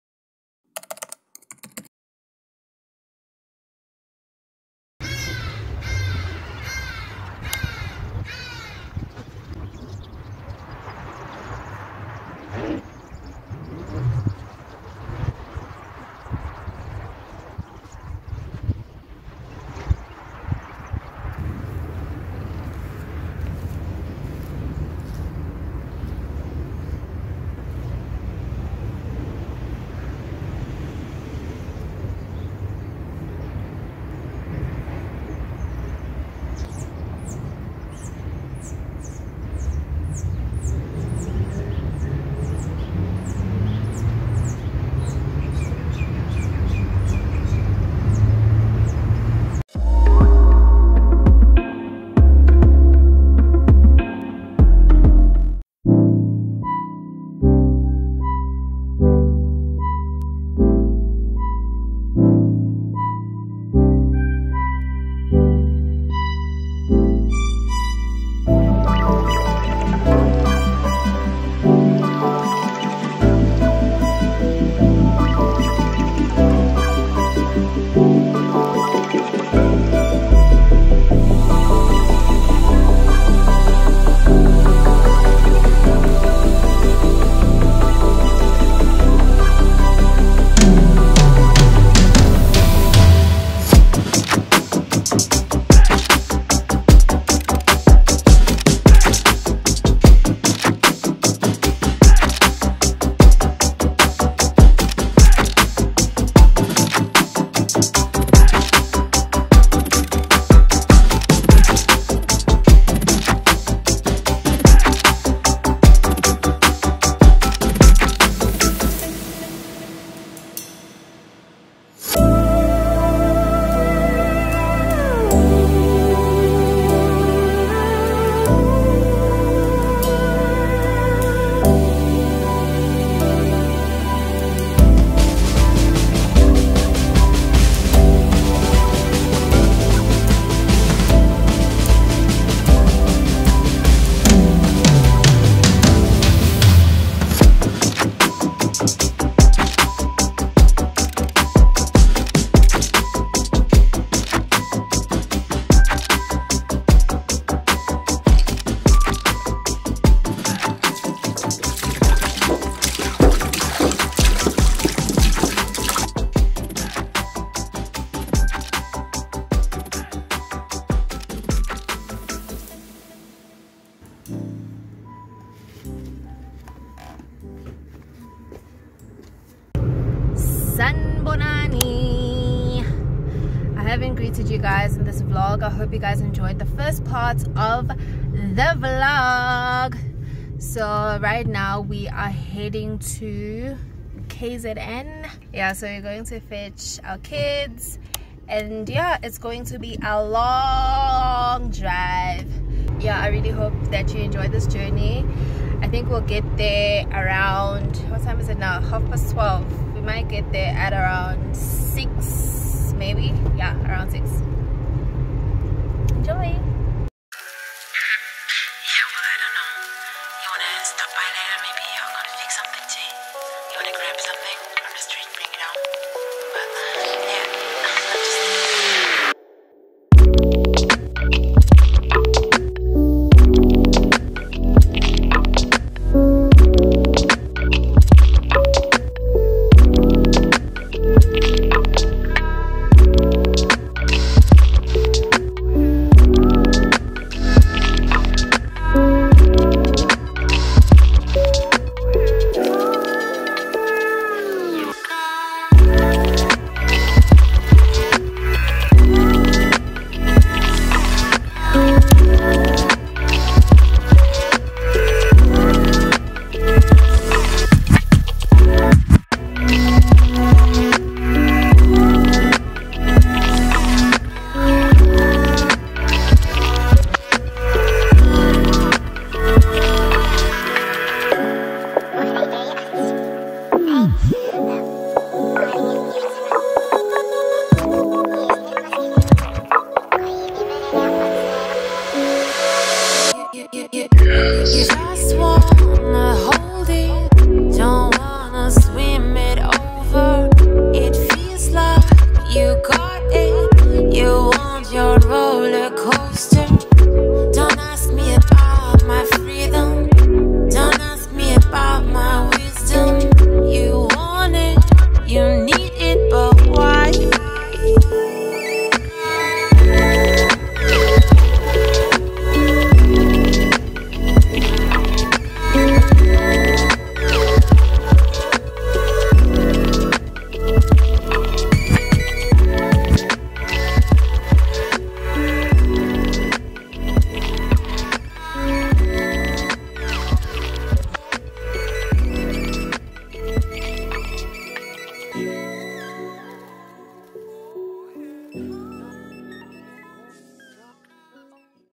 TRAVEL VLOG : ASCOT INN | PIETERMARITZBURG| SOUTH AFRICAN YOUTUBER
Hey BD babe.

Welcome back to another vlog, where I spent time with nature, and hearing of the birds and insects was beautiful, after a while we tend to forget outside does still exist. We then went to Pietermaritzburg to fetch the kids as they had spent almost the entire December with the grandparents, I must admit the rest was needed :) see you on my next one